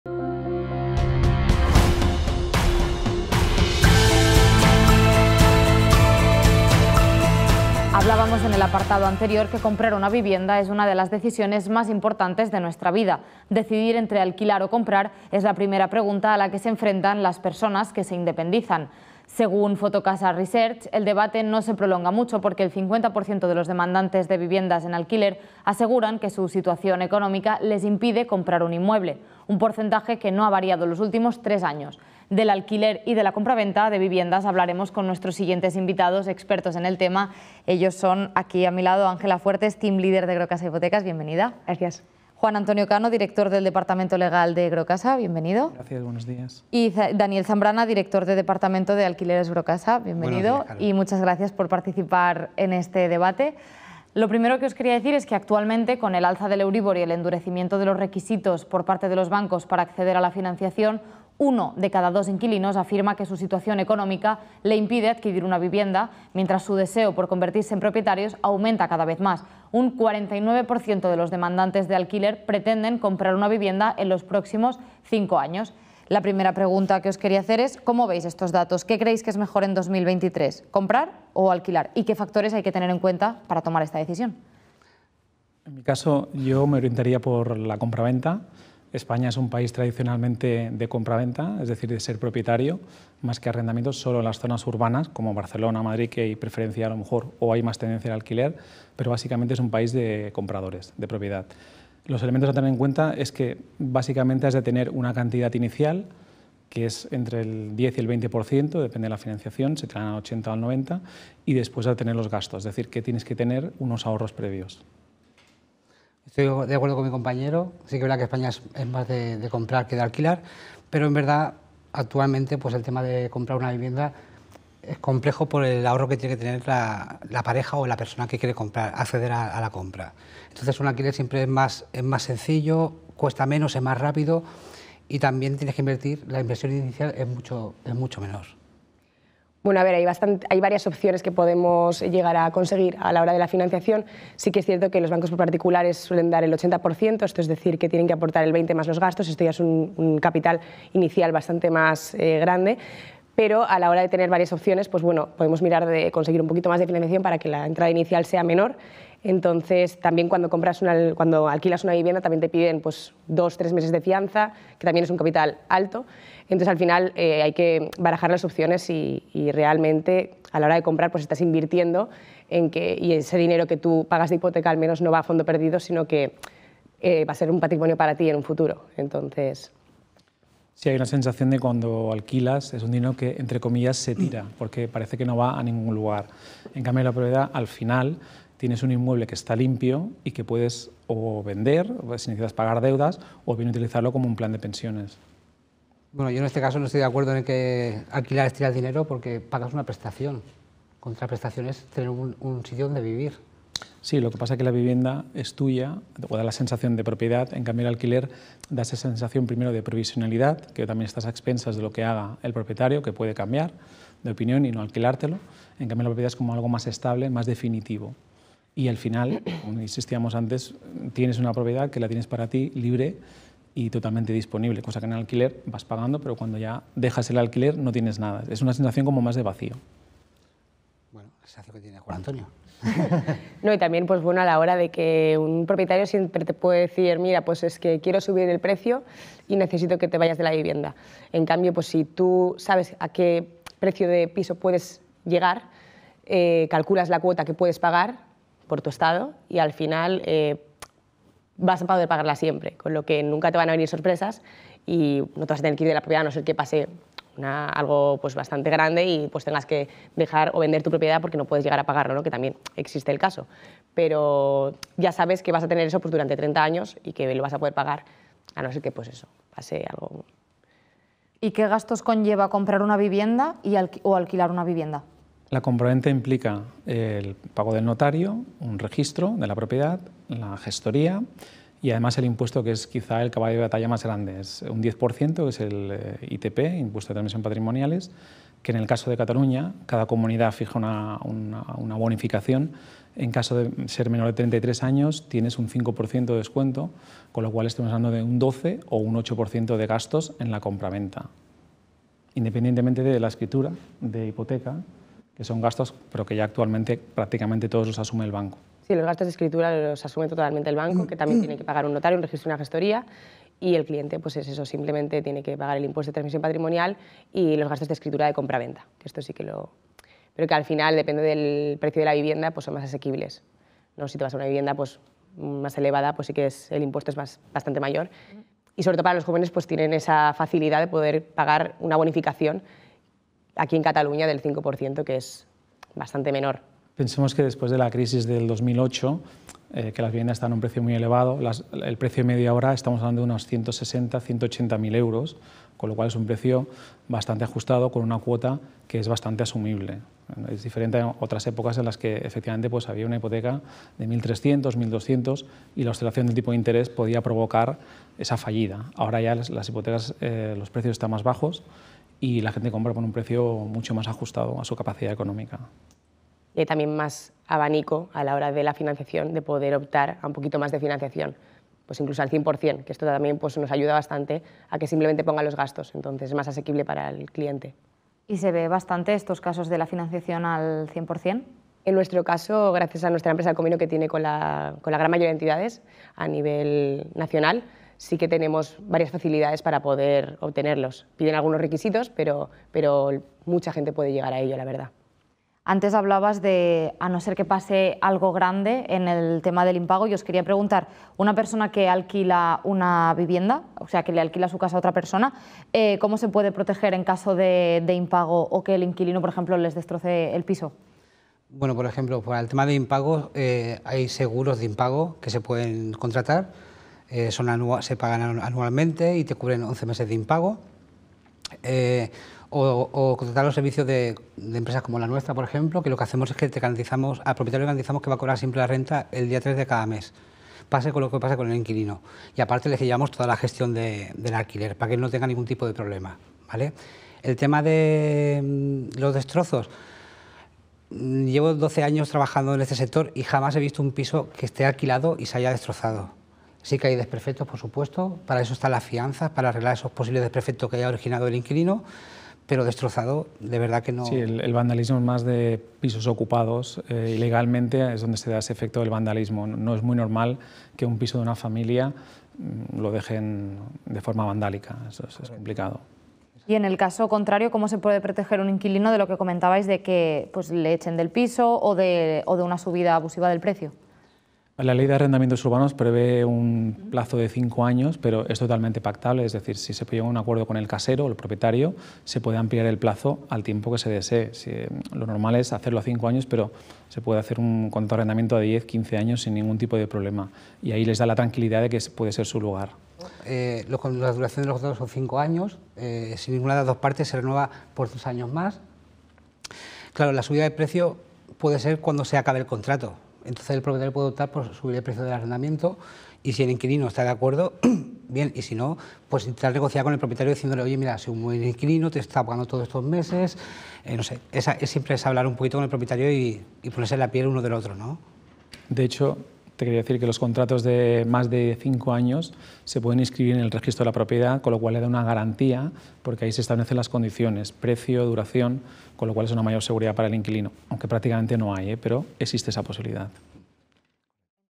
Comprar Vs Alquilar 🏠 ¿Qué es mejor?
Actualmente, con el alza del euríbor y el endurecimiento de los requisitos por parte de los bancos para acceder a la financiación hipotecaria, 1 de cada 2 inquilinos afirma que su situación económica le impide adquirir una vivienda, mientras su deseo por convertirse en propietarios, aumenta cada vez más. Un 49% de los demandantes de alquiler pretenden comprar una vivienda en los próximos cinco años.

🏠 ¿Comprar o alquilar? ¿Qué te conviene más?
💰¿Qué gastos se relacionan con el alquiler? ¿Y con la compraventa?
📝¿Cómo se protegen los arrendadores y los inquilinos?

0:00 Inicio
01:23 ¿Qué factores hay que tener en cuenta para alquilar o comprar?
04:40 ¿Qué gastos conlleva?
06:50 ¿Cómo se puede proteger el arrendador?
07:50 ¿Cómo se protege un inquilino?

De estos y otros temas relacionados con el sector hipotecario debatirán, en un nuevo episodio de Hablando con Crédito, los siguientes especialistas:

➡ Ángela Fuertes, Team Leader de Grocasa Hipotecas
➡ Juan Antonio Cano, Director del Departamento Legal de Grocasa
➡ Daniel Zambrana, Director Departamento Alquileres Grocasa